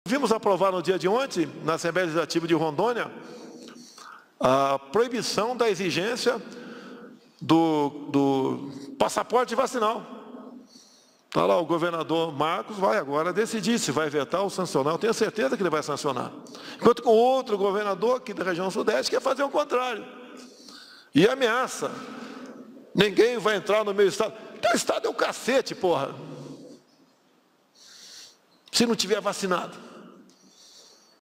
Bolsonaro PERDE A PACIÊNCIA com Doria e desabafa!
A competência constitucional para decidir sobre a entrada ou permanência de estrangeiros é FEDERAL.
E o Estado de São Paulo não é propriedade particular de um certo governador.
Nossa equipe já está trabalhando nas medidas para derrubar mais esse absurdo.